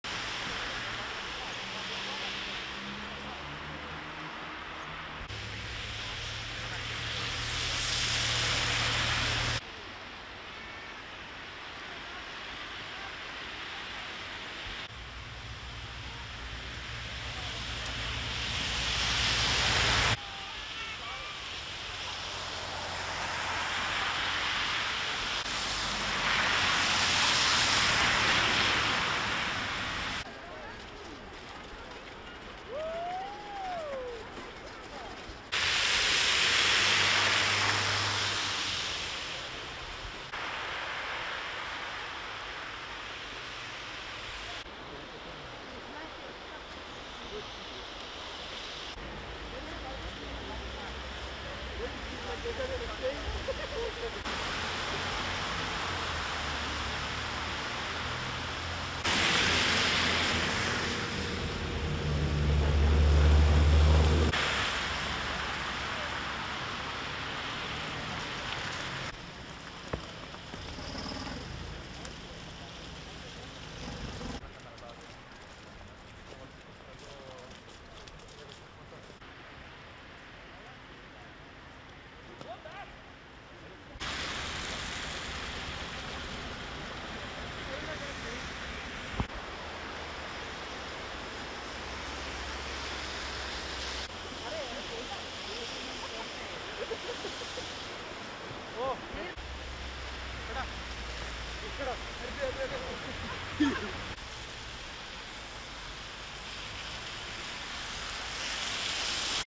Pavia, la città imbiancata dalla neve
La neve caduta nella notte e fino alla tarda mattinata di lunedì ha imbiancato le strade, i monumenti e le sponde del Ticino: così appare Pavia ai passanti e agli automobilisti nelle ore della prima nevicata d'inverno.